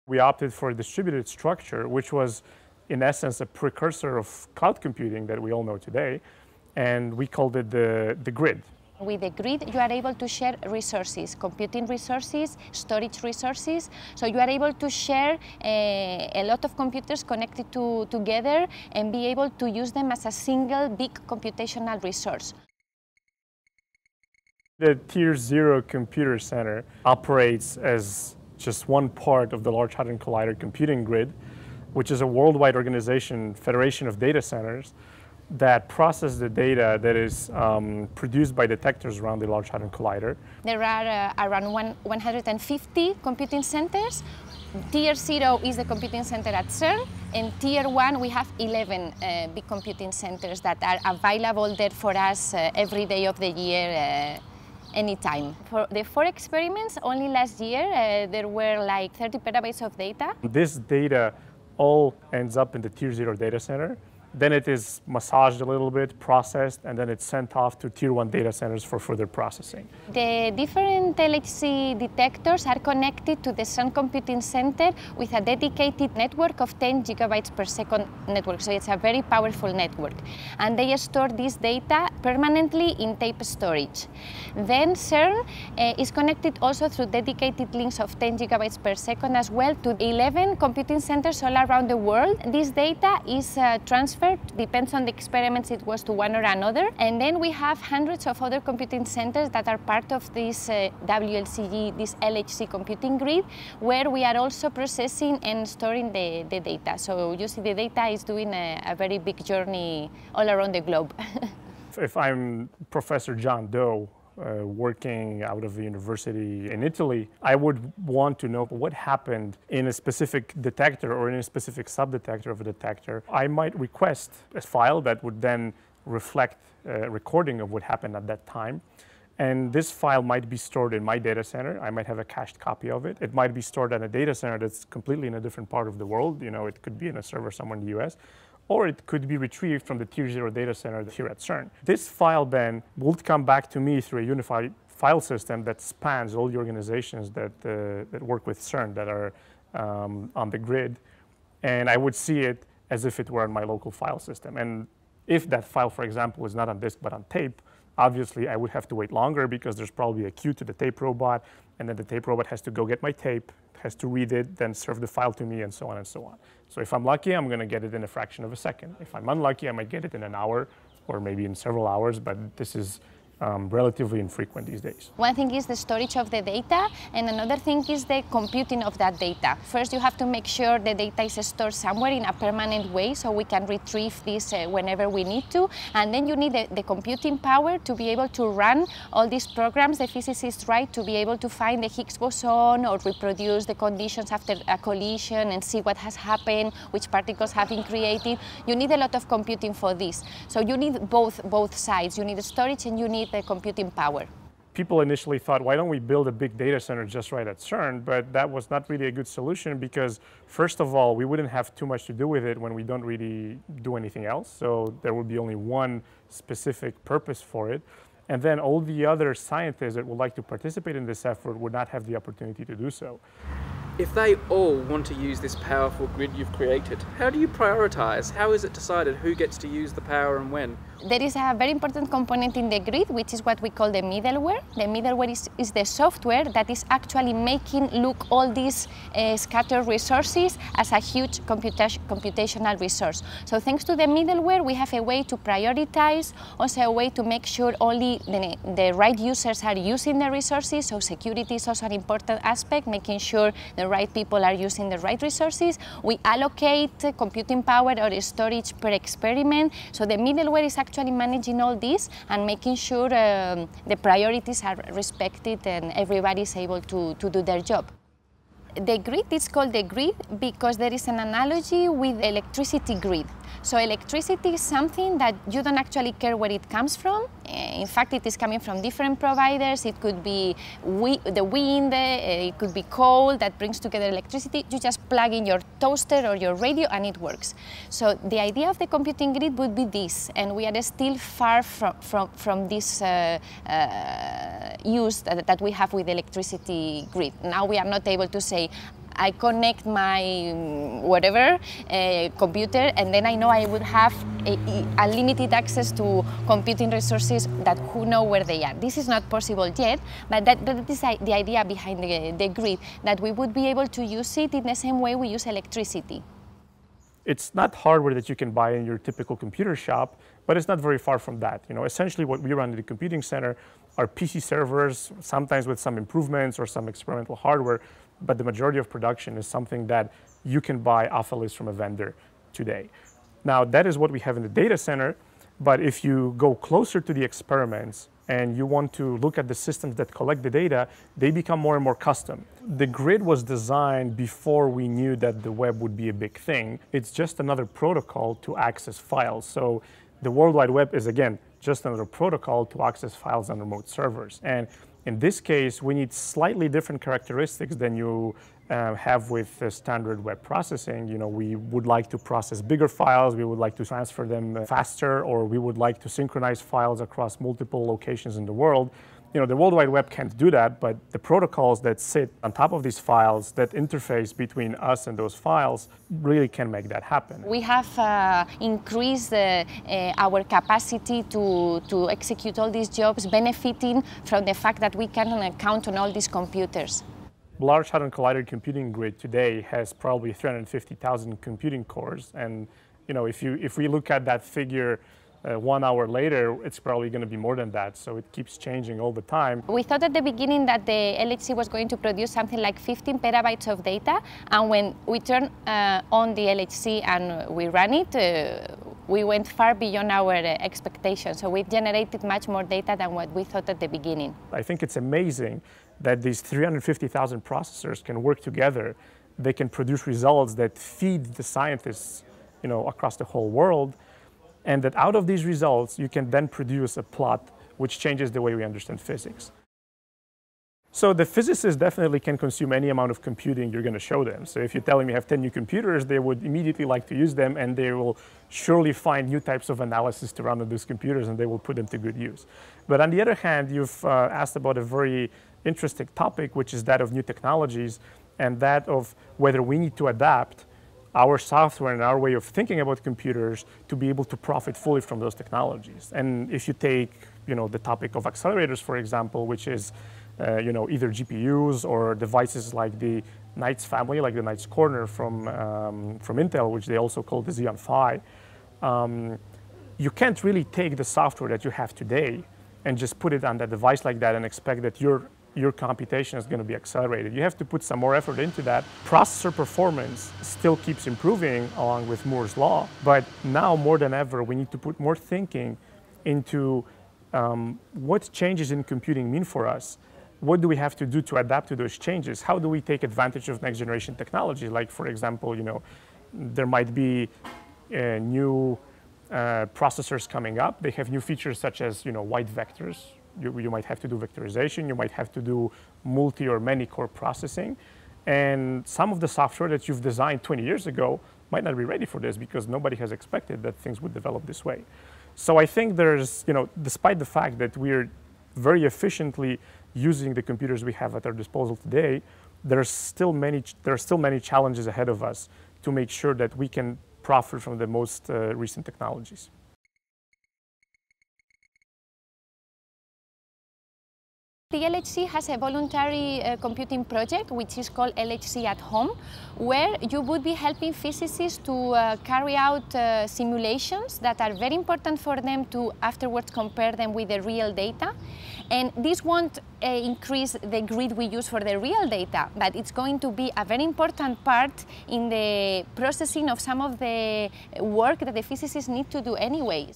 The Grid, CERN's Global Supercomputer - Computerphile
CERN developed their 'Grid' before the world wide web took off. Maria and Andrzej explain how it does a lot more than just share the reams of data the Large Hadron Collider collects.

Correction: at 1min 16s, Maria meant to say 10 Gigabits Per Second (not gigabytes)

Maria Alandes Pradillo - Grid Information System expert
Andrzej Nowak - CERN Openlab CTO Office

This video was edited by Sean Riley.

Computerphile is a sister project to Brady Haran's Numberphile. See the full list of Brady's video projects